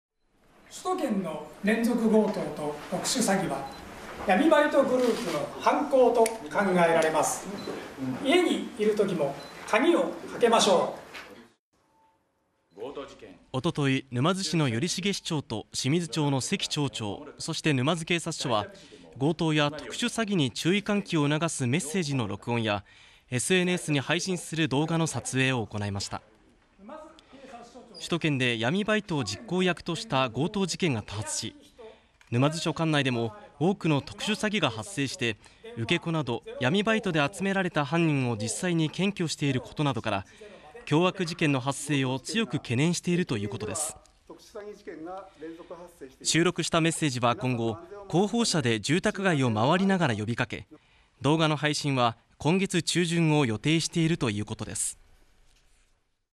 【被害防止】強盗や特殊詐欺事件が多発する中…自治体と警察が協力しＳＮＳで配信する啓発動画を撮影(静岡)
首都圏で強盗事件も相次ぐ中、静岡･沼津市で5日、「強盗や特殊詐欺」の被害を防ごうと、SNSに配信する動画の撮影が行われました。